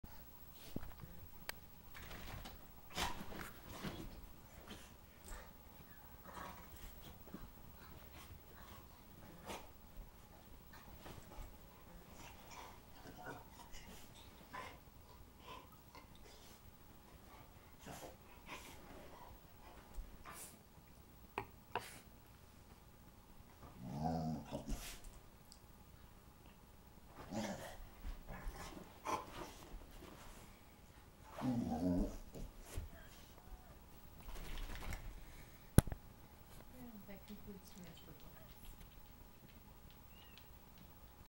Aggie attacks her bed, 4/19/09
Our English Springer Spaniel Aggie assaults her bed once, sometimes twice, each night. Inspired by arekdreyer's dinner videos of Phoebe, I've started keeping the Flip handy to try to capture her acting all possessed and crazy.